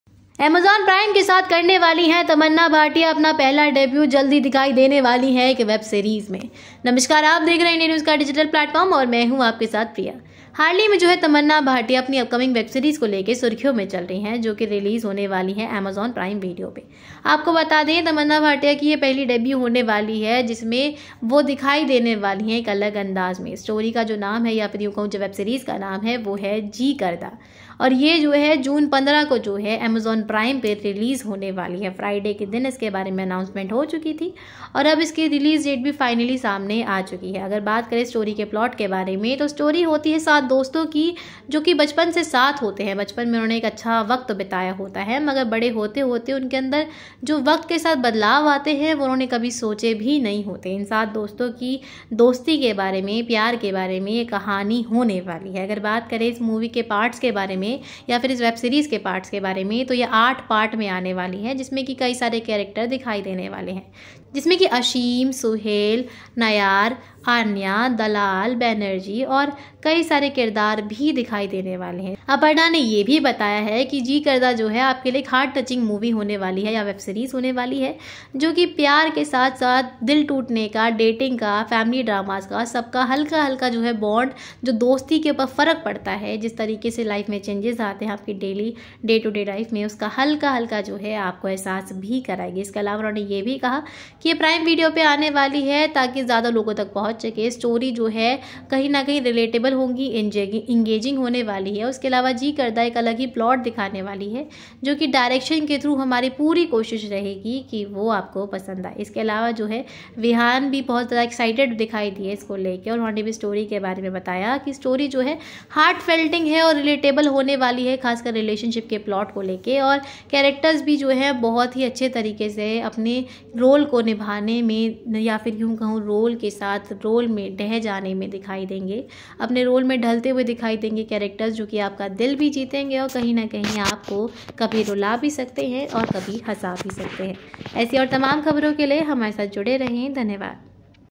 Jee Karda OTT Release Date: Tamannaah Bhatia की Upcoming Web Series इस OTT Platform पर होगी Release
बॉलीवुड की जानी मानीं एक्ट्रेस तमन्ना भाटिया अपकमिंग वेब सीरीज ‘जी करदा‘ को लेकर चर्चा में हैं। फिल्म जल्द ही ओटीटी पर रिलीज होने वाली है जिसके लिए तारीख तय हो गई है। फिल्म को Prime Video पर रिलीज किया जाएगा। इस सीरीज में बचपन के दोस्तों की कहानी दिखाई गई है। यह एक रोमांस ड्रामा फिल्म है जिसमें दिखाया गया है कि बचपन से लेकर बड़े होने तक ये दोस्त किस तरह से जिंदगी के अलग अलग पहलुओं से परिचित होते हैं।
Bollywood's well-known actress Tamannaah Bhatia is in discussion about the upcoming web series 'Jee Karda'. The film is soon going to release on OTT for which the date has been fixed. The film will be released on Prime Video. The story of childhood friends is shown in this series. It is a romantic drama film that shows how these friends are exposed to different aspects of life from childhood to growing up.